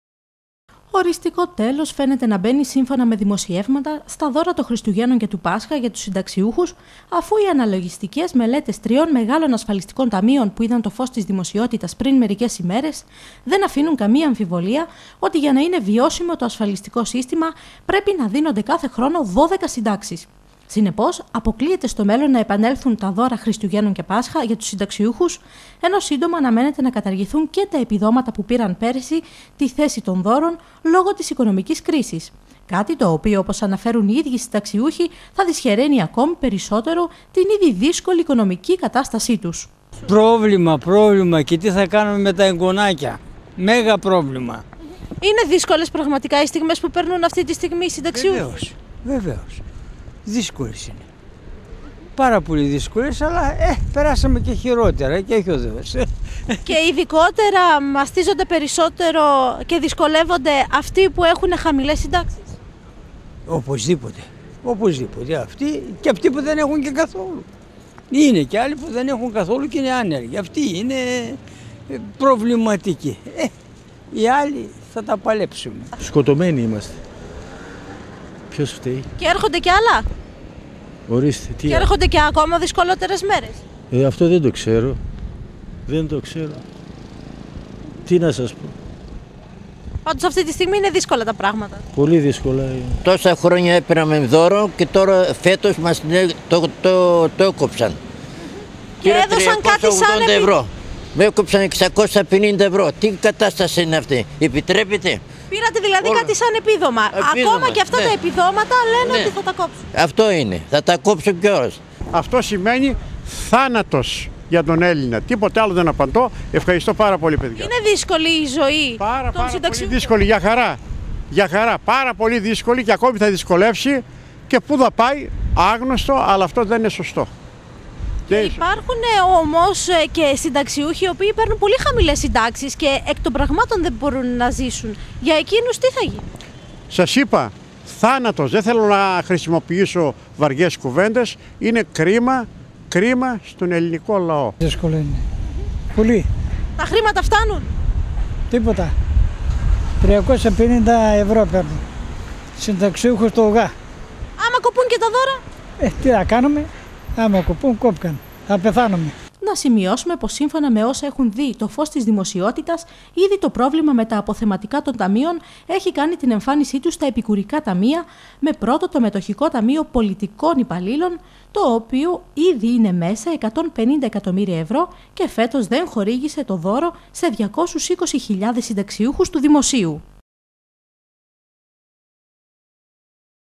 ΕΡΧΟΝΤΑΙ ΑΚΟΜΗ ΠΙΟ ΔΥΣΚΟΛΕΣ ΜΕΡΕΣ...
Κανάλι 6 - ΕΡΧΟΝΤΑΙ ΑΚΟΜΗ ΠΙΟ ΔΥΣΚΟΛΕΣ ΜΕΡΕΣ ΓΙΑ ΤΟΥΣ ΣΥΝΤΑΞΙΟΥΧΟΥΣ 28-1-11